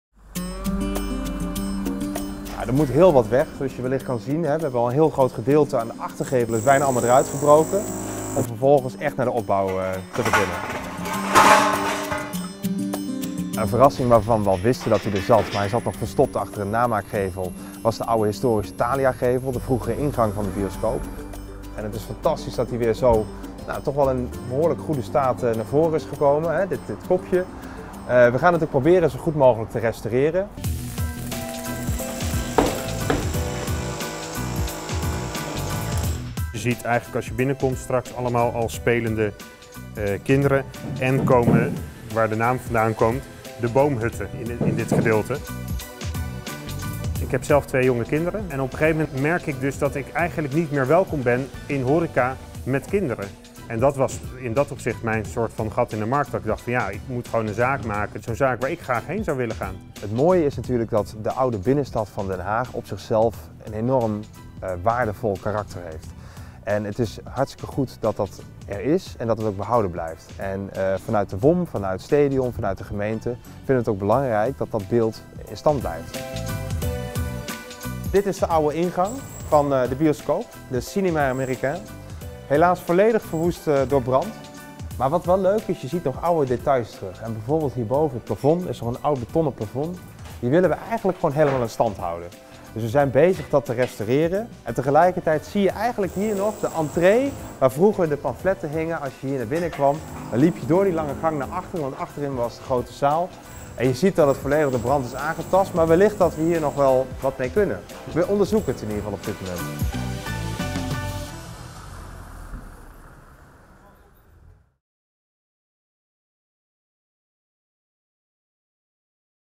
Van Broekenhuispand naar Boomhuttenclub: een kijkje achter de schermen
Staedion renoveert het oude Broekenhuispand aan de Boekhorststraat. Medio april 2018 komt hier De Boomhuttenclub daarboven een aantal appartementen. Ooit was dit pand bekend als Cinema Americain en later de Thalia bioscoop met een historische gevel.